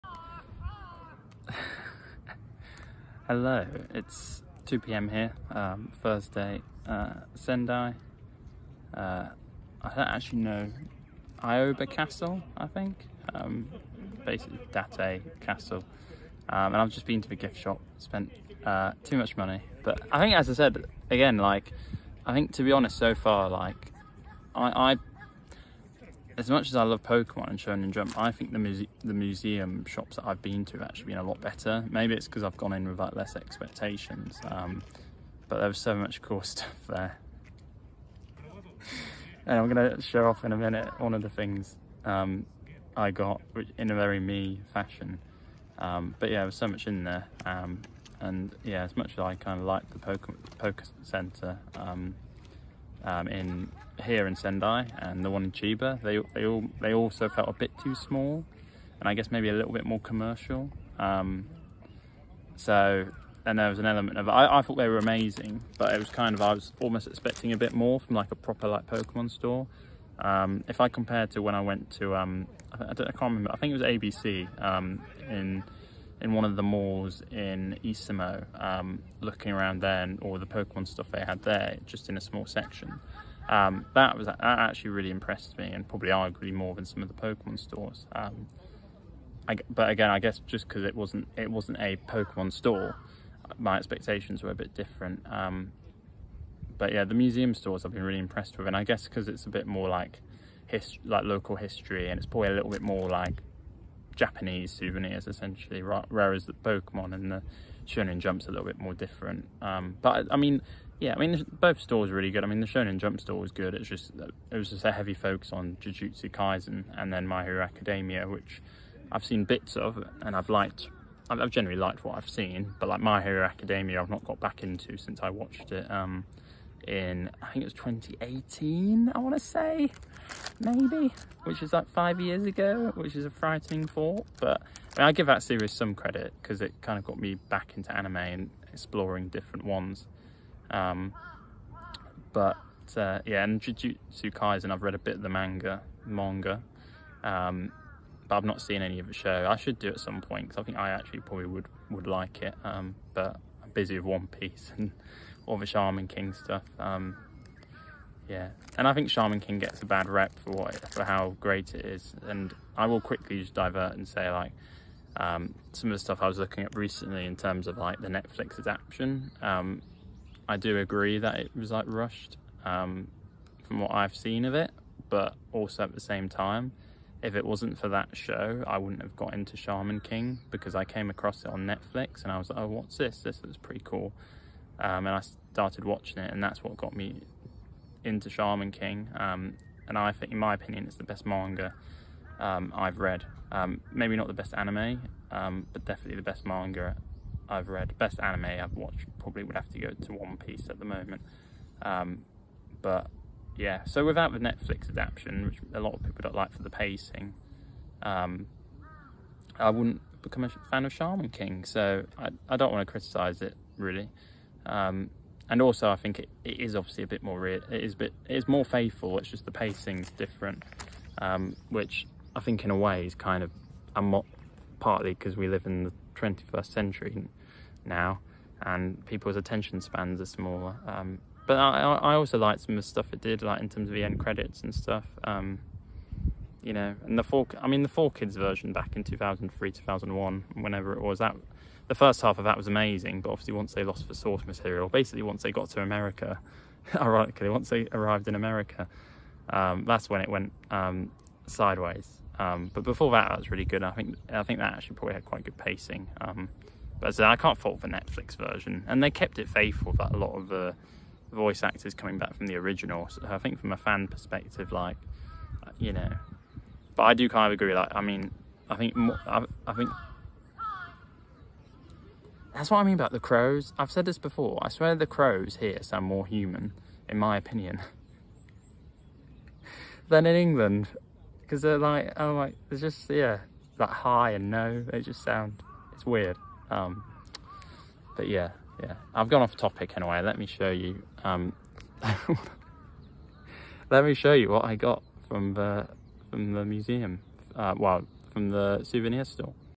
329 Best Gift Shop I've been to in Japan? (Aoba Castle Ruins (Sendai) Date Masamune)
329 Best Gift Shop I've been to in Japan? (Aoba Castle Ruins (Sendai) Date Masamune)

Video captured on 1st June 2023.